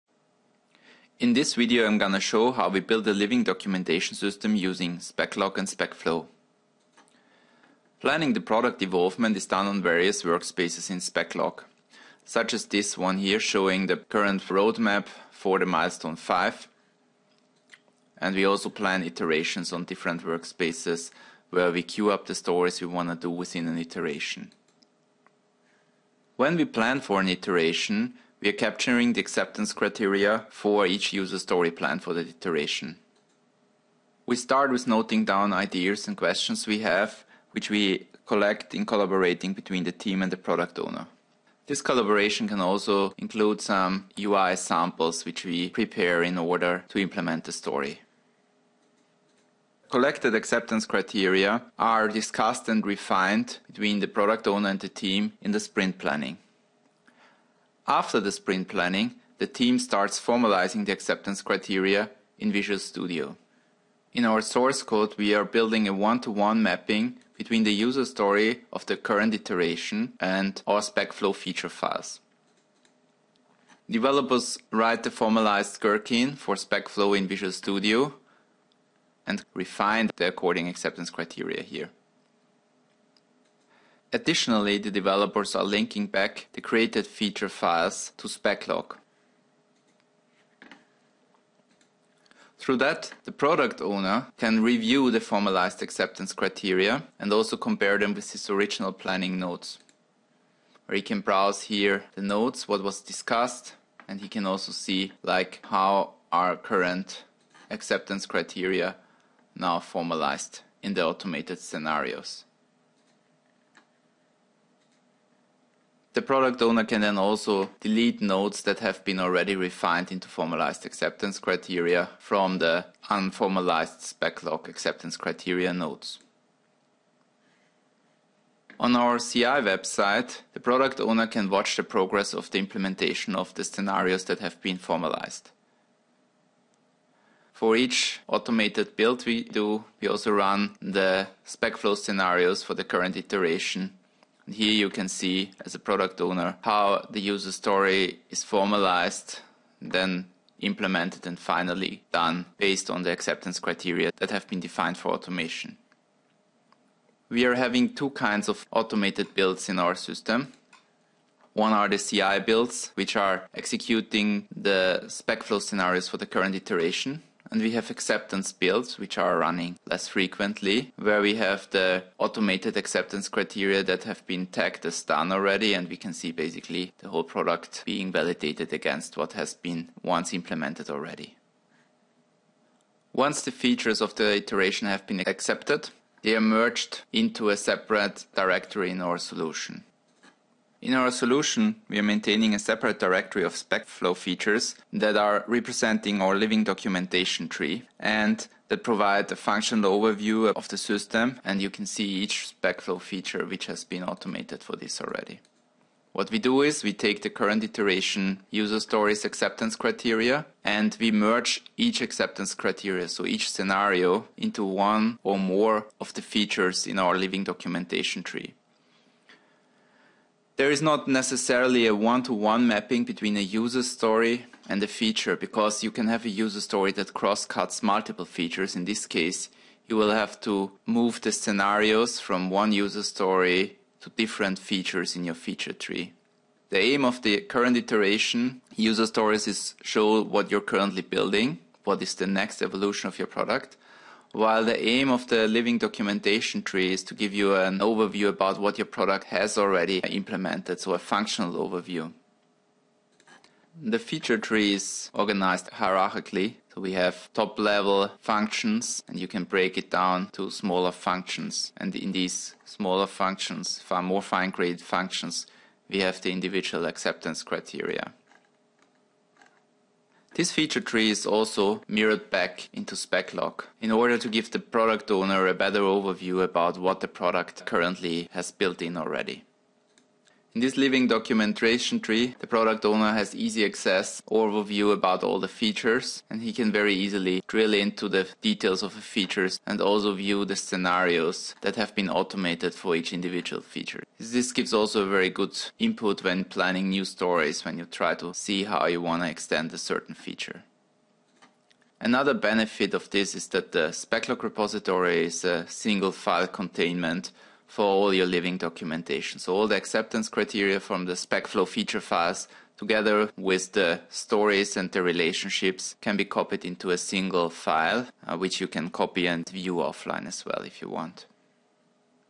Building a living documentation system with SpecLog and SpecFlow
Shows how we build a living documentation system using SpecLog and SpecFlow in our own product development.